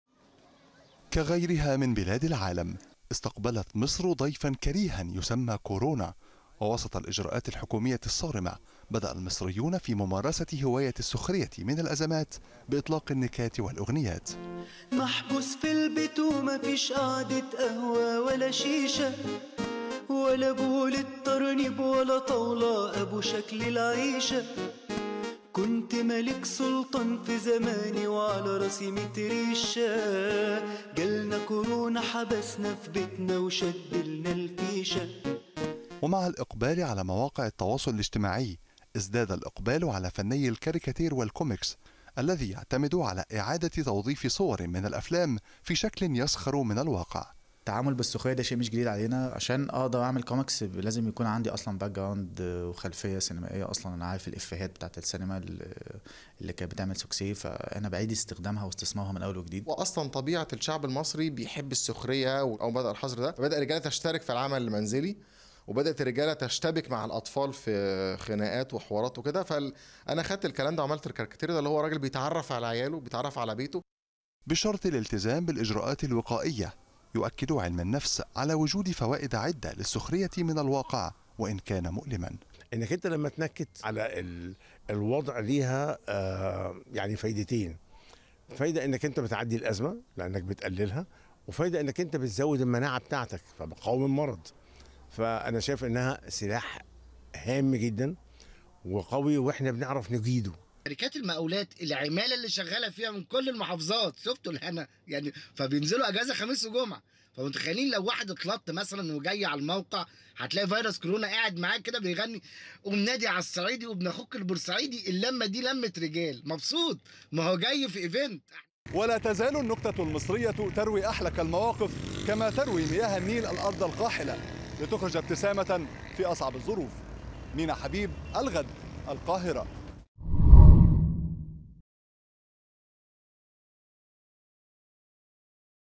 النكتة المصرية تروي أحلك المواقف.. شاهد كيف استقبل المصريون أزمة فيروس كورونا
القاهرة - الغد: عرف الشعب المصري بأنه من أكثر الشعوب العربية إنتاجا وتداولا للنكتة والسخرية من الواقع.
ولا يقتصر إنتاج وتبادل النكات على المواقف الطريفة فقط، بل يلجأ إليها المصريون وسط الأزمات لهذا وجد في حظر التجوال والاجراءات الاحترازية ضد كورونا مادة خصبة للنكات.
المزيد من التفاصيل في سياق تقرير مراسل الغد في القاهرة مينا حبيب عبر النشرة الإخبارية.